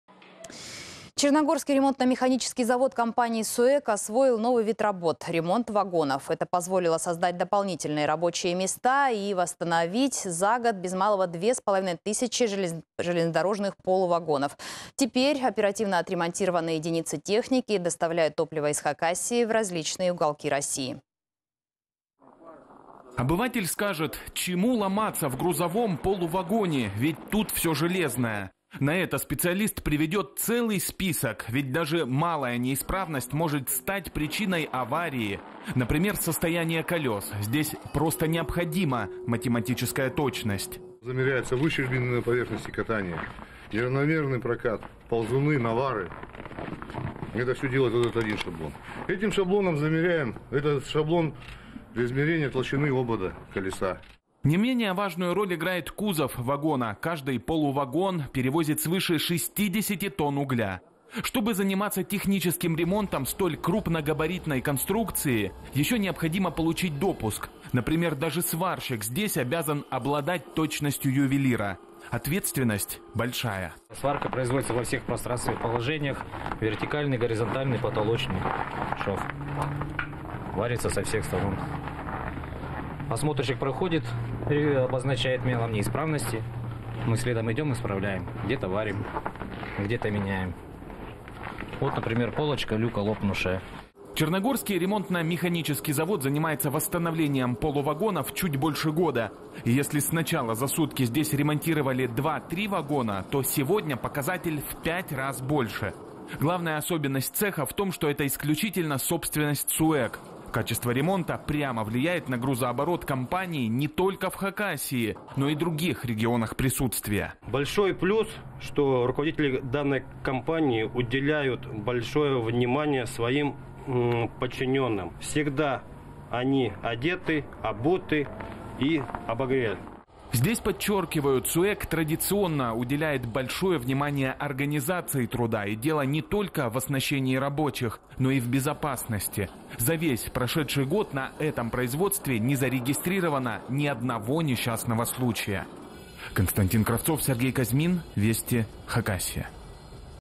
«Черногорский РМЗ» компании СУЭК освоил новый вид работ – ремонт вагонов. 23.01.2019
Это позволило создать новые рабочие места для жителей Хакасии и произвести за год ремонт без малого двух с половиной тысяч железнодорожных полувагонов. Благодаря заводчанам тысячи оперативно отремонтированных полувагонов доставили в различные уголки России топливо из Хакасии.